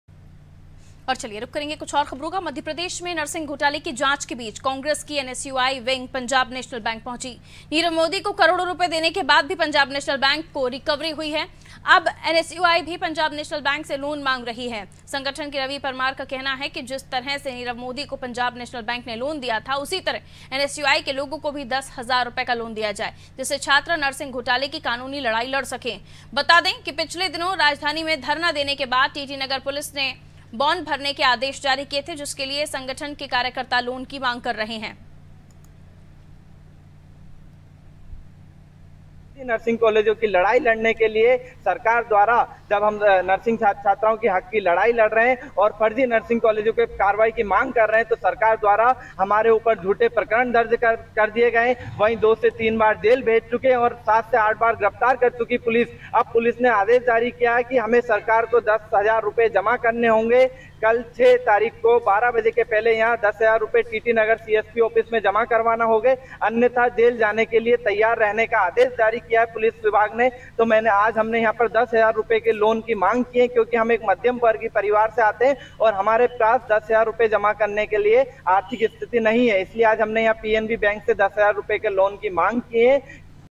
Bhopal : NSUI मांग रहा लोन, पंजाब नेशनल बैंक से मांगा लोन || Anaadi Tv
मध्यप्रदेश में नर्सिंग घोटाले की जांच के बीच कांग्रेस की एनएसयूआई विंग पंजाब नेशनल बैंक पहुंची... नीरव मोदी को करोड़ों रुपए देने के बाद भी पंजाब नेशनल बैंक को रिकवरी हुई है. अब एनएसयूआई भी पंजाब नेशनल बैंक से लोन मांग रही है... संगठन के रवि परमार का कहना है कि जिस तरह से नीरव मोदी को पंजाब नेशनल बैंक ने लोन दिया था... उसी तरह एनएसयूआई के लोगों को भी 10 हजार रुपए का लोन दिया जाए. जिससे छात्र नर्सिंग घोटाले की कानूनी लड़ सकें... बता दें कि पिछले दिनों राजधानी में धरना देने के बाद टीटी नगर पुलिस ने बांड भरने के आदेश जारी किए थे... जिसके लिए संगठन के कार्यकर्ता लोन की मांग कर रहे हैं...

COMING SOON
 DEN, AIRTEL